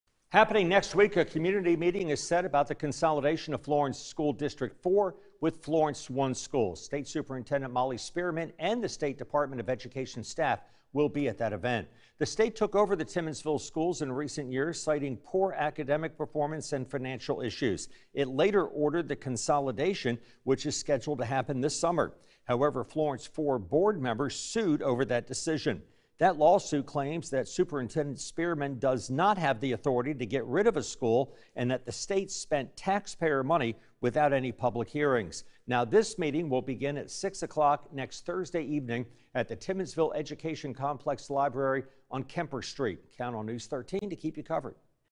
Molly Spearman, South Carolina Department of Education to hold community meeting over Florence 4 con
Molly Spearman, South Carolina Department of Education to hold community meeting over Florence 4 consolidation